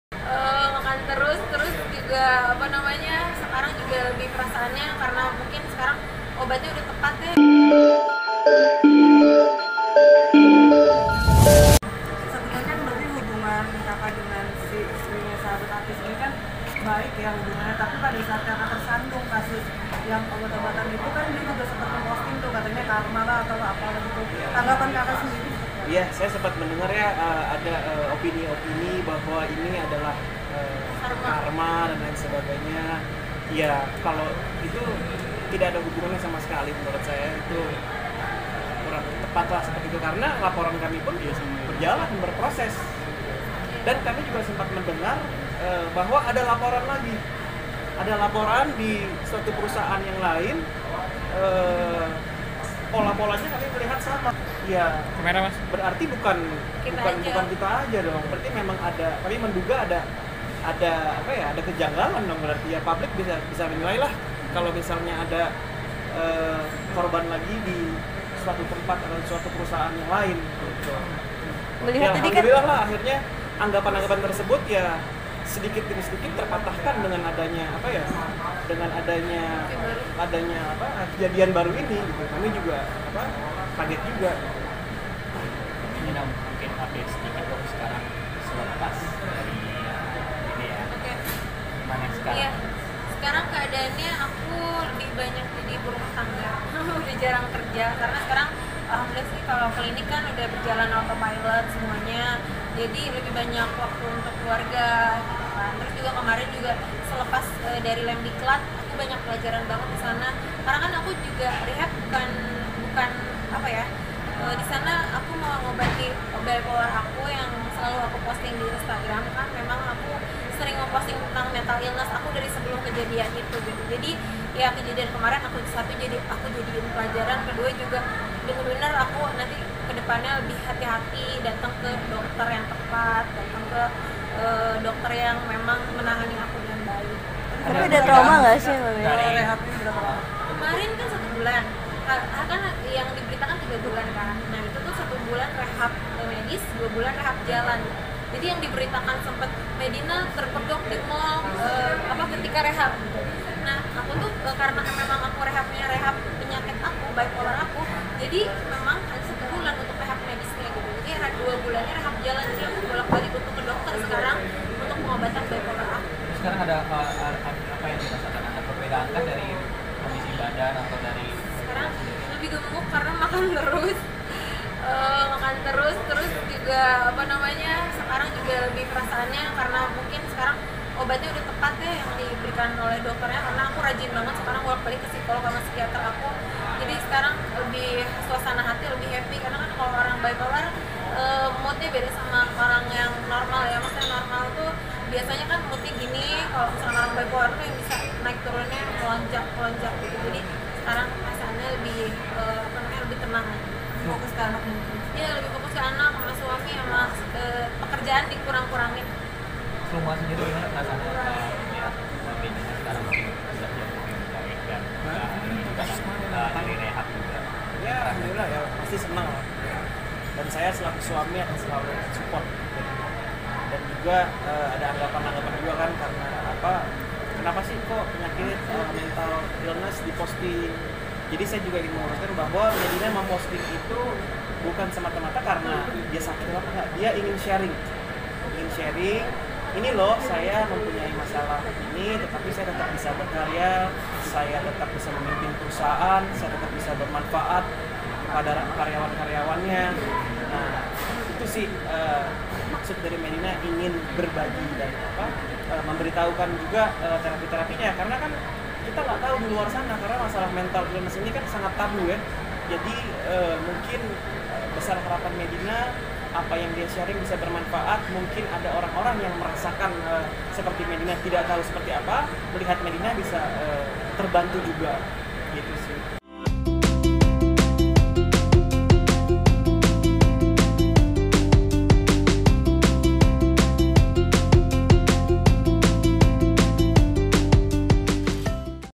Medina Zein Kurangi Aktivitas Bekerja, Ini yang Dirasakannya
Pasca peristiwa yang dialaminya, Medina Zein kini mengurangi aktivitasnya dalam bekerja. Ia lebih memilih fokus mengurus buah hati dan suami. Kepada wartawan, Selasa (17/3) di kawasan Jakarta Selatan, ia mengungkapkan kini lebih merasa segar. Bagaimana berita selengkapnya? (Kadir/Beepdo.com)